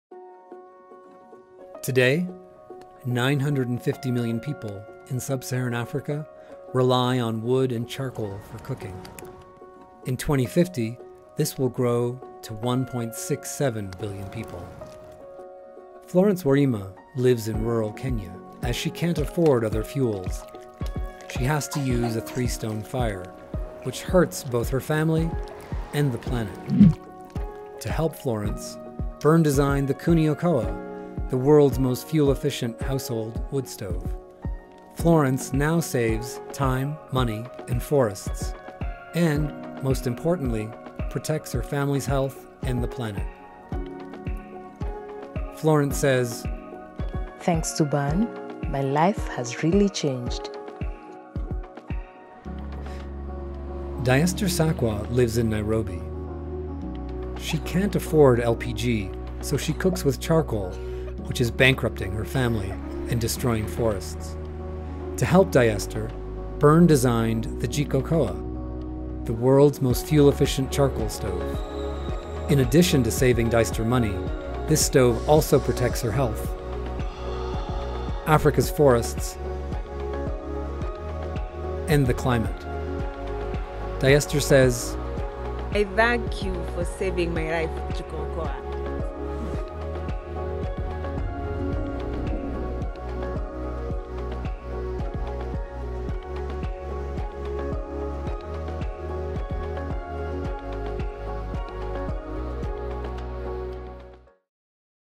Why Biomass Stoves Matter - Now More Than Ever!
Biomass currently provides ~85% of all household cooking energy in Sub-Saharan Africa.

Due to population growth alone, an additional 75,000 people on the continent rely on biomass each day.

Although BURN now offers a line of electric, hybrid and liquid fuel stoves, we believe it is imperative to provide super fuel-efficient solutions to households that only have access to biomass fuels. Watch the video to learn more about our approach.